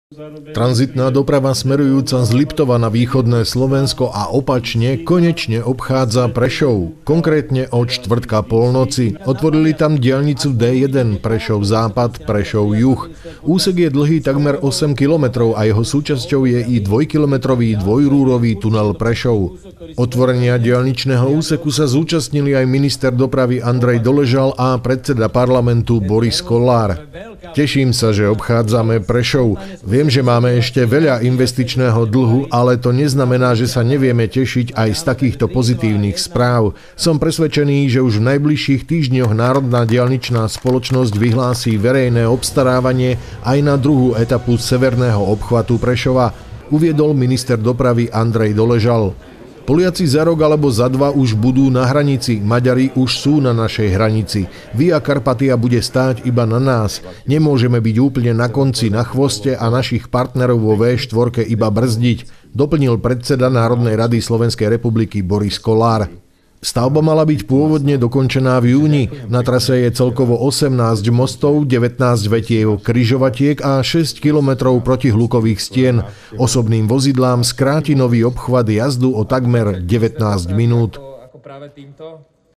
TV Romana - Prešov konečne s obchvatom
Otvorili západný obchvat Prešova, motoristom ušetrí 19 minút.
Tento úsek je súčasťou obchvatu Prešova, je spoplatnený a treba na ňom diaľničnú známku. Rýchlostná cesta R4 bude tvoriť „severný“ obchvat mesta.